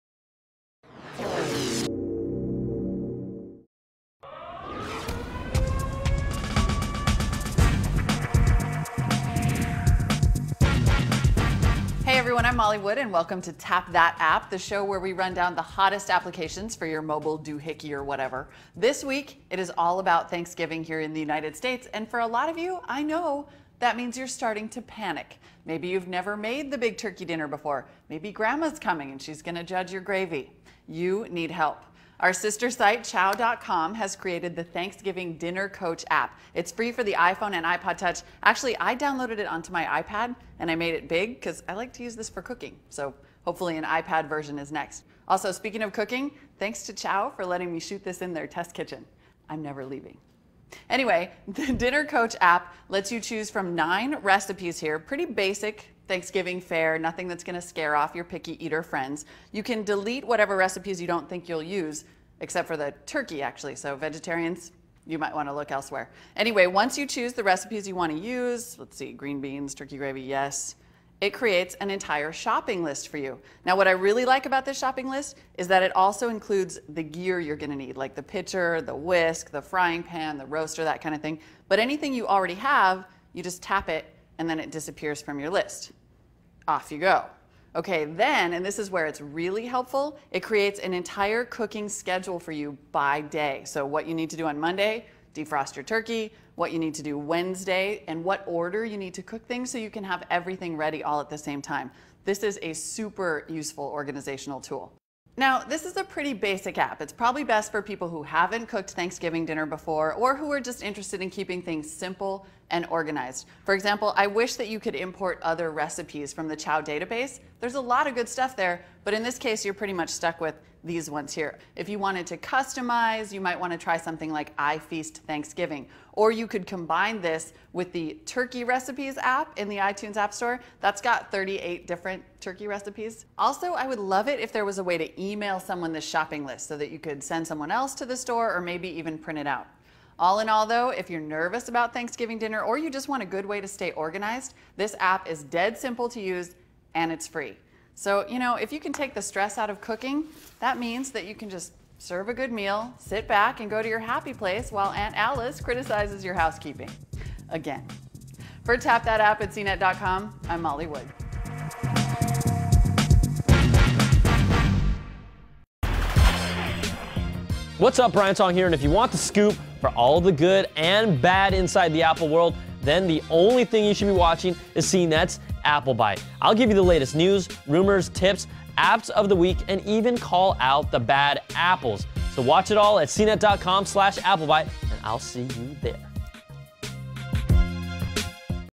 Tap That App: Chow Thanksgiving Dinner Coach
Don't panic! Preparing Thanksgiving dinner is a snap with this easy-to-use app that helps you stay organized while you plan, shop, and cook a delicious meal. And it's free.